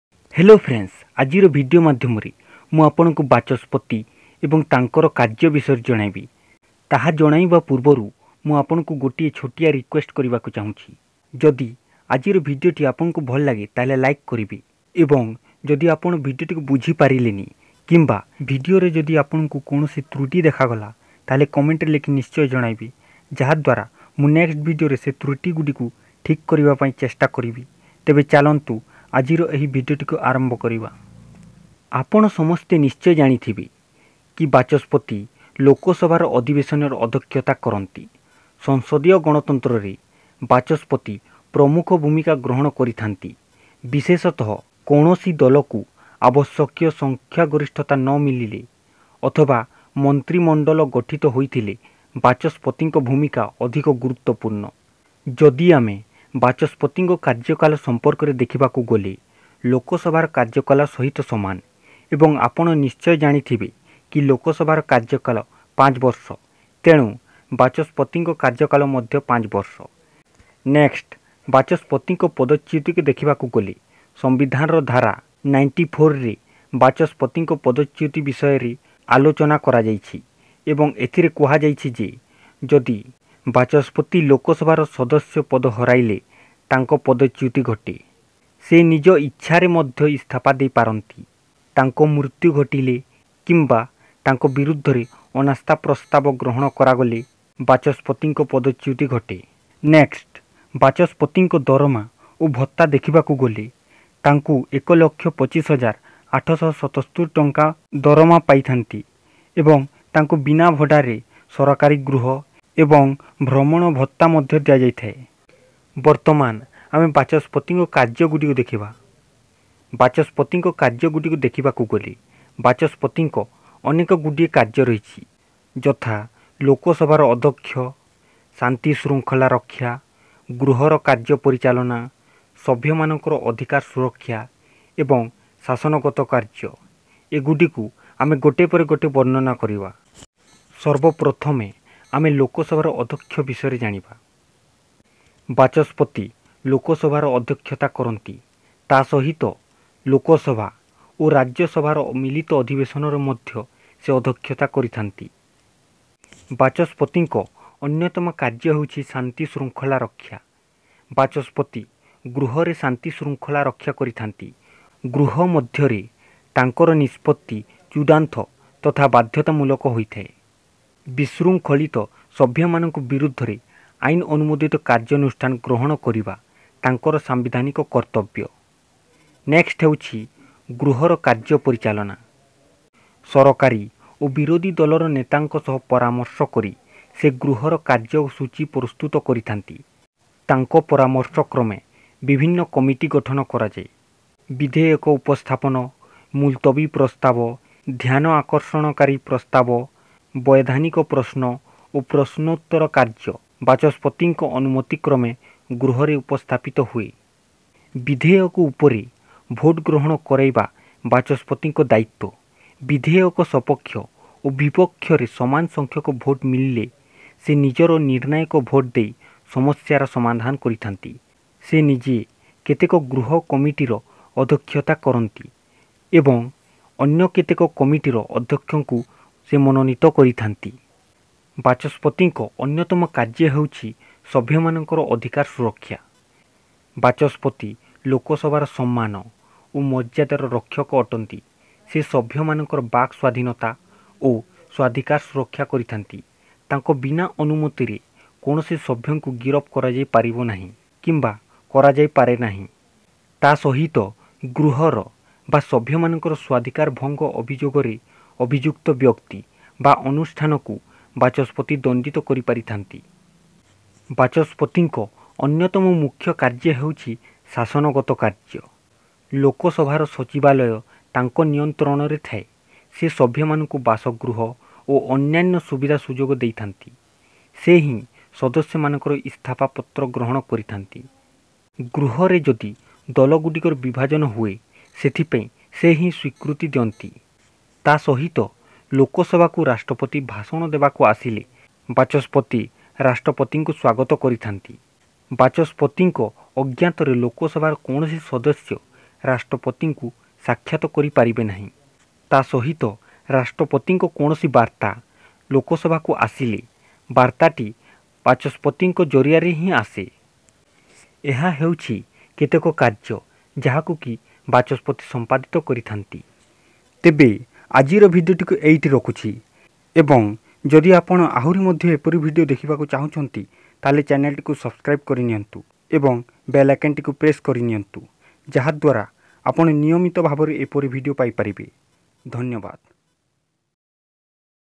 Speaker Of The Lok Sabha In Odia - ବାଚସ୍ପତି ( Powers and Functions )
ଏହି ଭିଡିଓ ମାଧ୍ୟମରେ ଆପଣ Speaker ବା ଲୋକସଭାର ବାଚସ୍ପତି ବିଷୟରେ ଜାଣିପାରିବେ । ତା ସହିତ ତାଙ୍କ Powers ଏବଂ Functions ବିଷୟରେ ମଧ୍ୟ ଜାଣିପାରିବେ ।